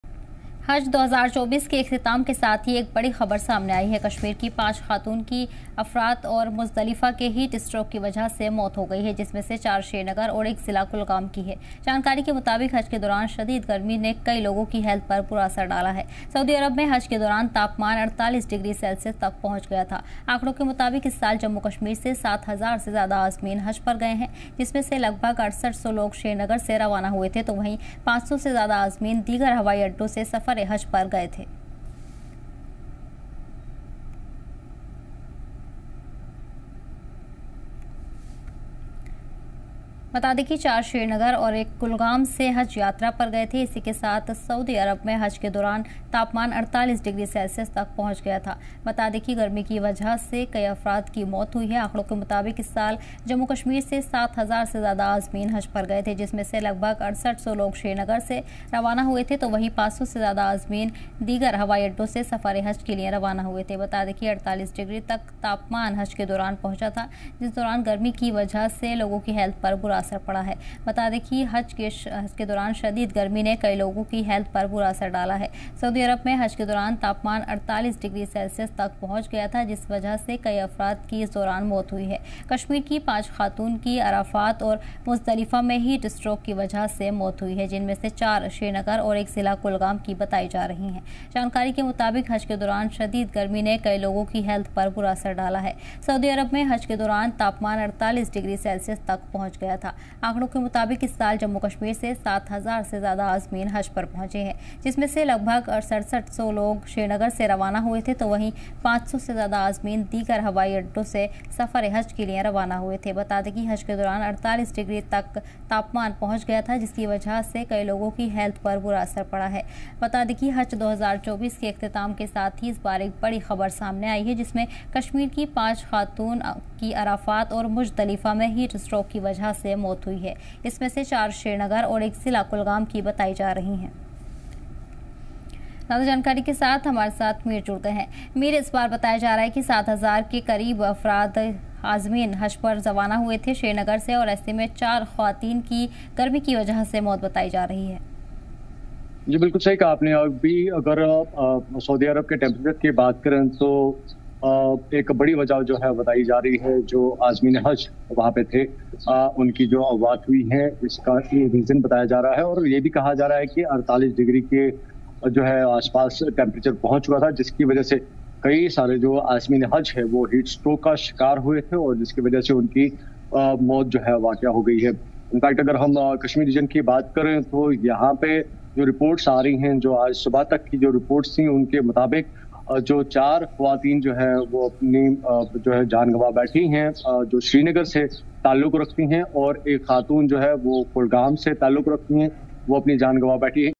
Hajj 2024 News : Hajj par gayi ghati ki khavatin ki loo se maut | Breaking News | Kesar TV
Hajj par gayi ghati ki khavatin ki loo se maut